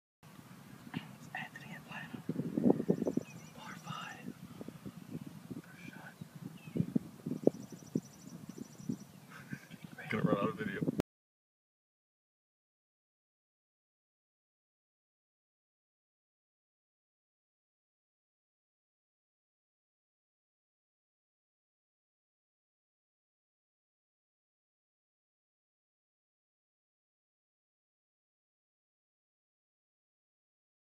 My Swing For 2013 Thanks Adams Golf For the New Driver Shaft
My  new swing for 2013 ,  same  driver different shaft .  we golded that day with danny batman paul Albany  goober Albany and tommy pga